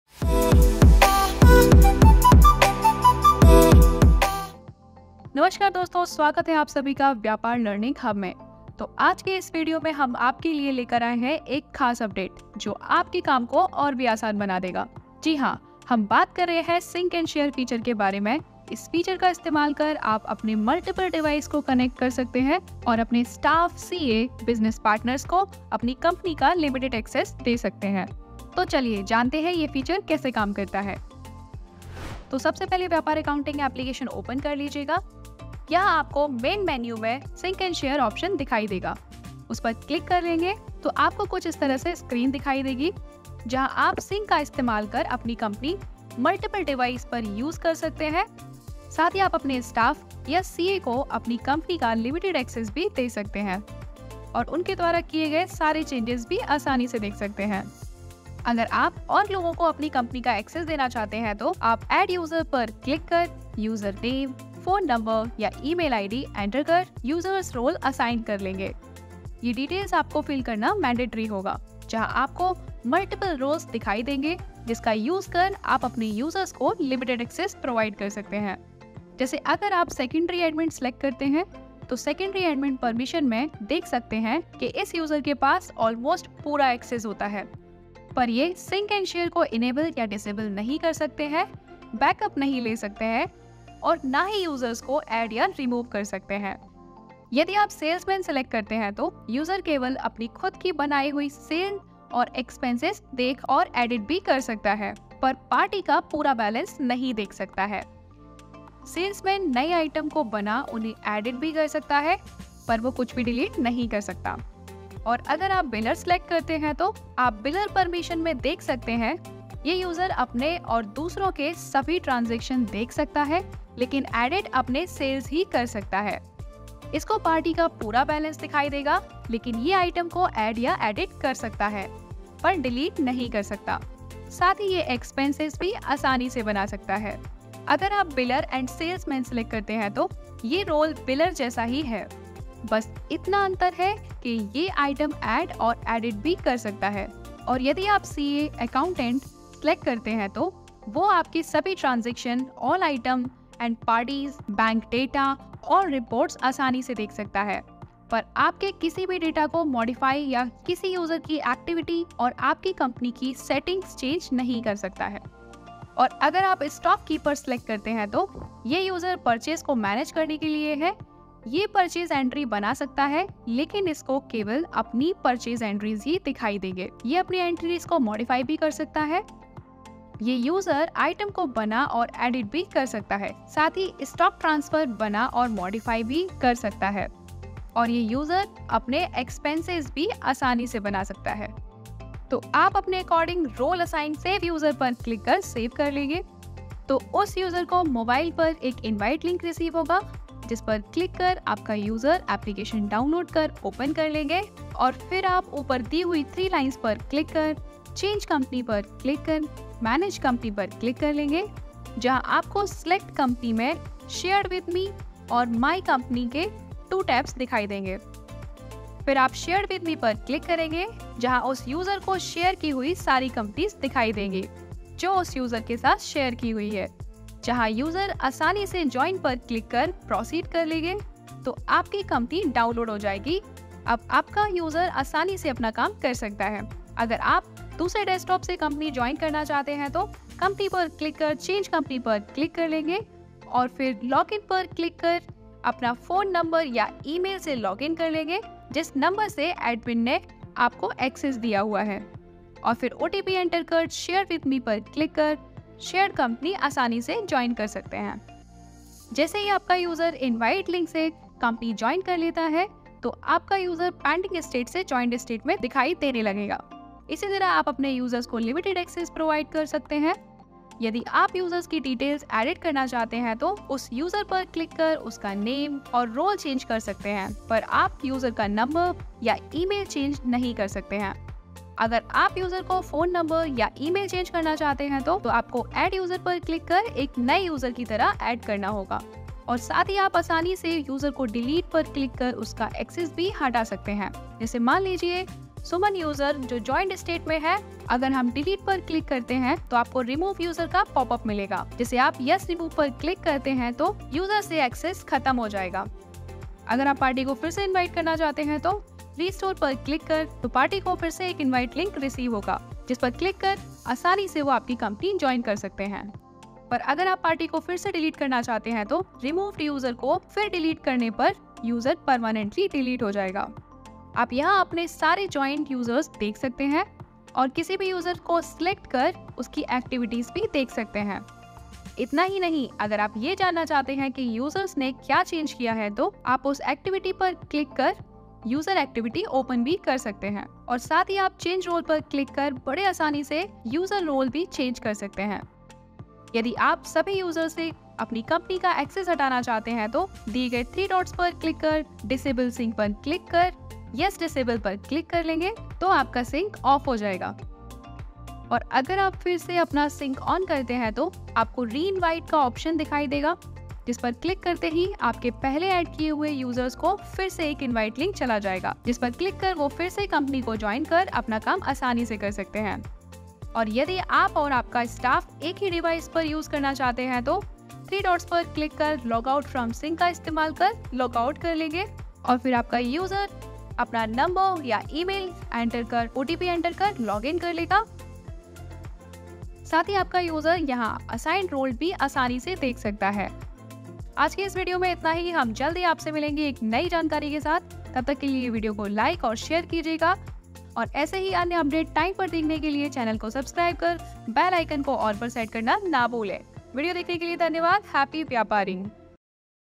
Multiple Devices लिंक करें और जुड़ें अपने Employees और Accountant से Limited access के साथ | Desktop
. मल्टी यूजर सिंक्रोनाइजेशन कर बिज़नेस को मल्टीपल डिवाइस पर एक साथ यूज़ करें, जानिये कैसे ? Isi ke sath ye bhi janange ki-
1. Sync and Share kaam kaise karta hai?
2. Aapke pass sabhi users ki details kaise dikhengi?
3. Sync and Share ka use kar ek device me multiple times login logout kese karein?

Dhyan rakhne wali baate-
1. Sync And Share ka istemal kar kitne bhi users ko access diya ja sakta hai? chahe vo mobile user ho ya desktop user.
2. Sync And Share premium or licensed devices mein hi kiya ja sakta hai.

Agar aap apne business ki billing, inventory management or accounting ko aasan banana chahte hai to abhi Vyapar download karein  😍